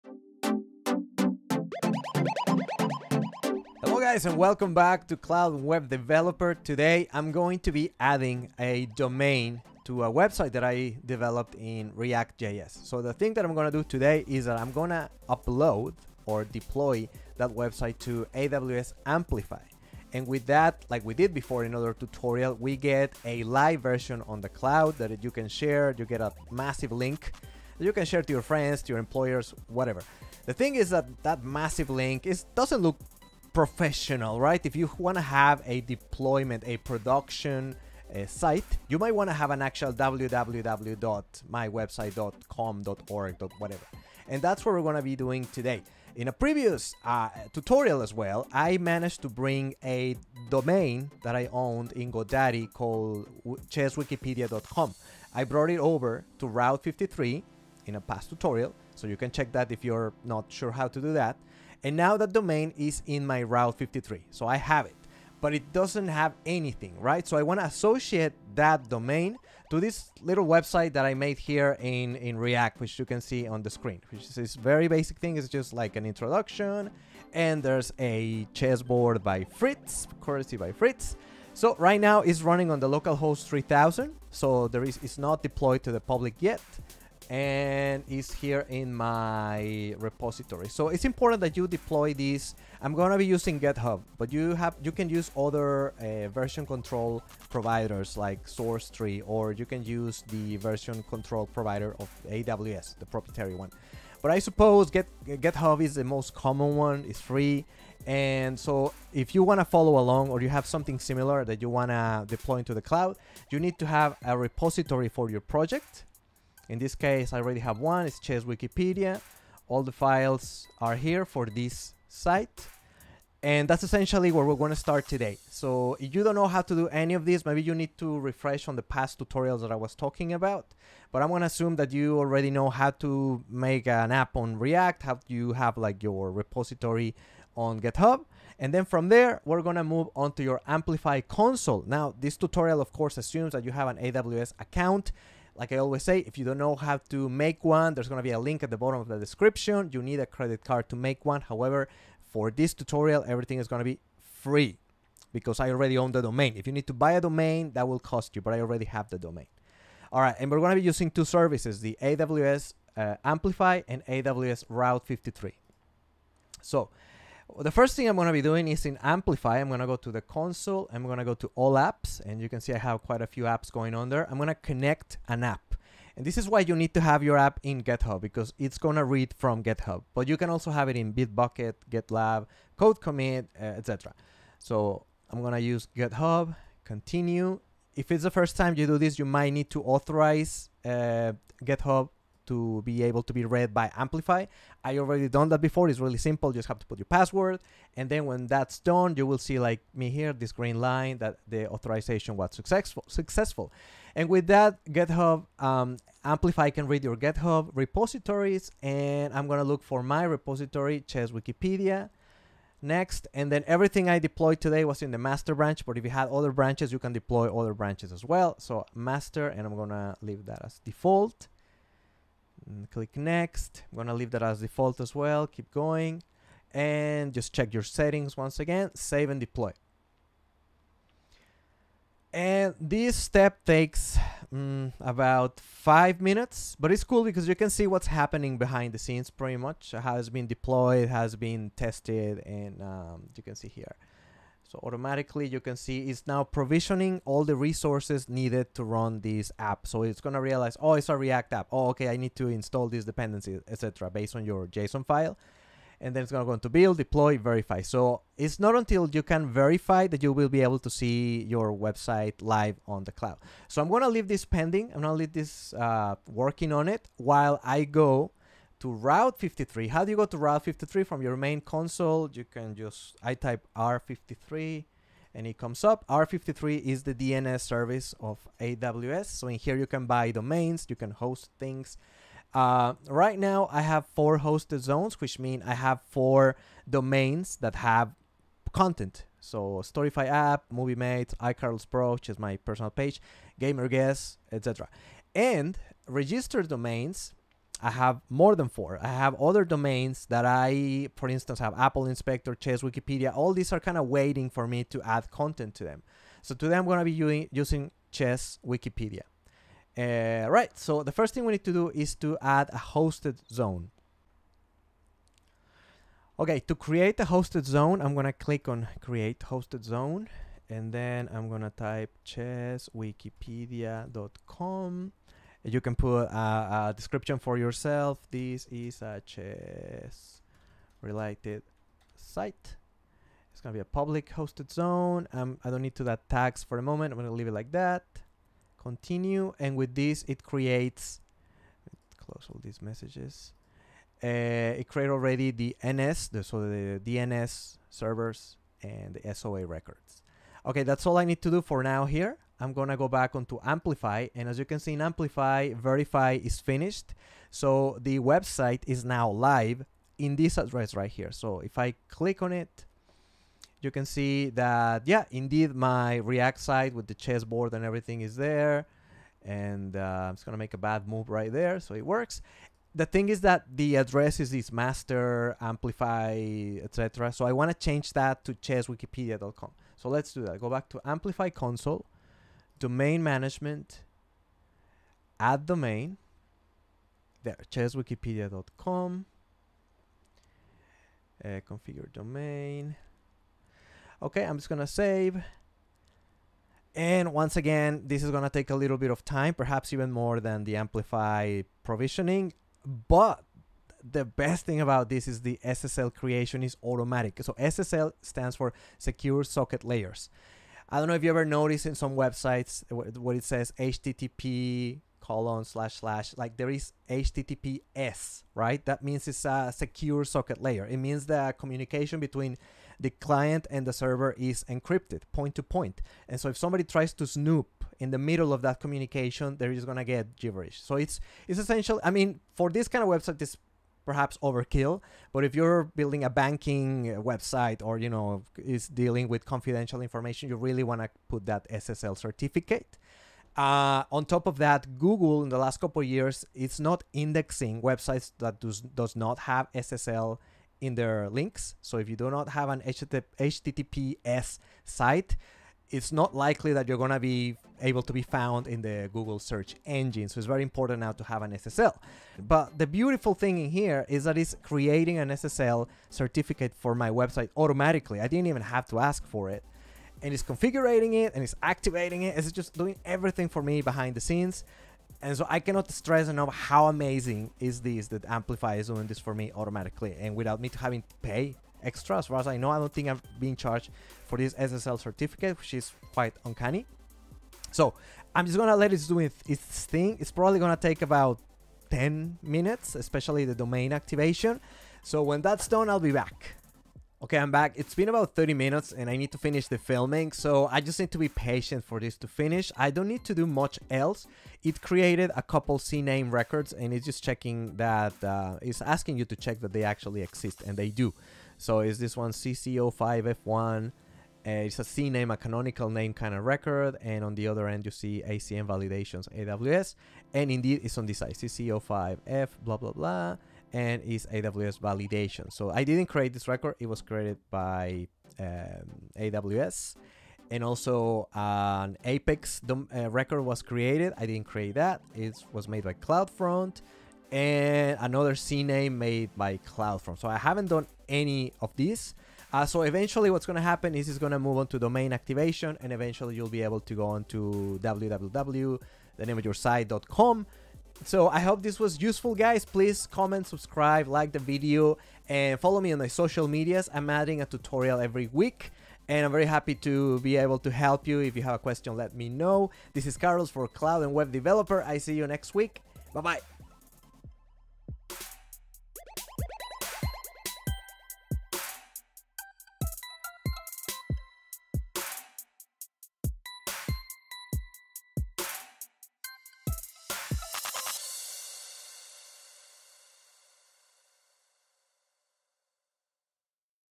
TUTORIAL - How To Assign a Domain Name to WebApp in AWS Amplify & Route 53 with SSL.
This is a tutorial that will take you step by step how to assign a Domain name you already own in Route 53 to an App that we will deploy in AWS Amplify.

Please refer to my previous tutorials to learn how to make a React App and deploy to AWS.  Also check my video about bringing over domain names from Godaddy to Route 53.

*Correction: In the video I mentioned this wouldn't cost you anything but it will cost you $0.50USD per month per hosted zone.

How to make an AWS account: